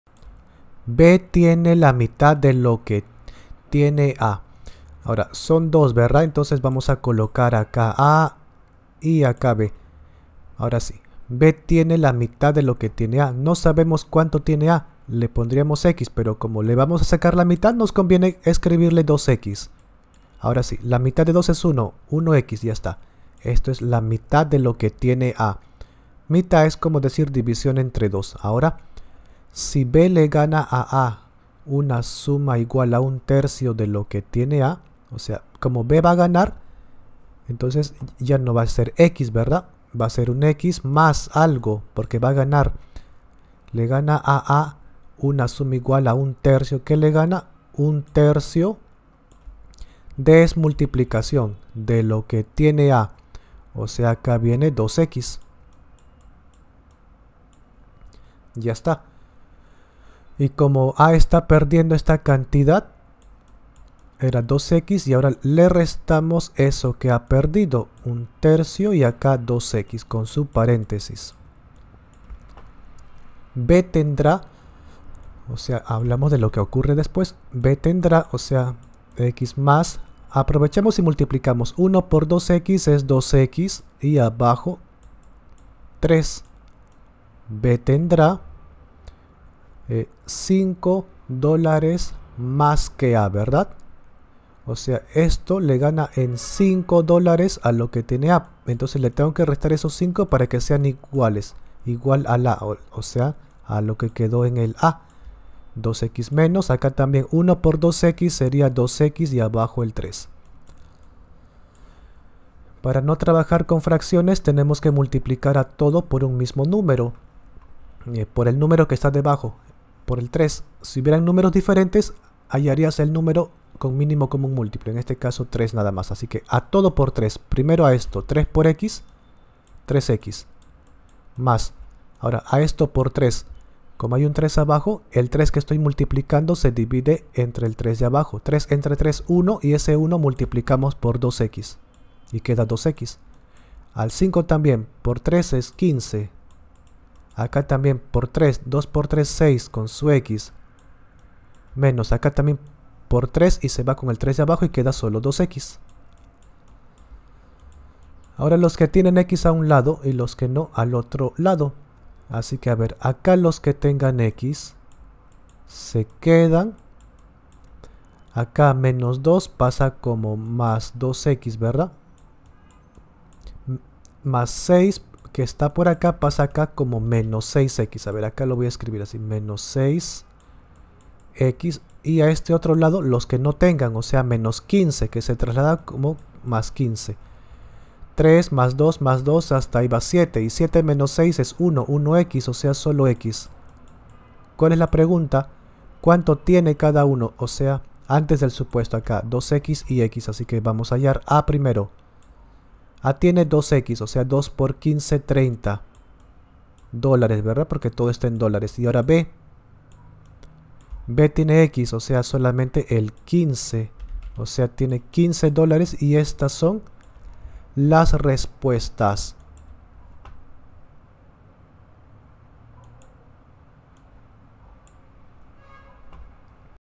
B tiene la mitad de lo que tiene A Si B le gana a A una suma igual a 1/3 de lo que tiene A B tendrá
B tiene la mitad de lo que tiene A. Si B le gana a A una suma igual a 1/3 de lo que tiene A, B tendrá $  5 más que A . ¿Cuánto tiene cada uno?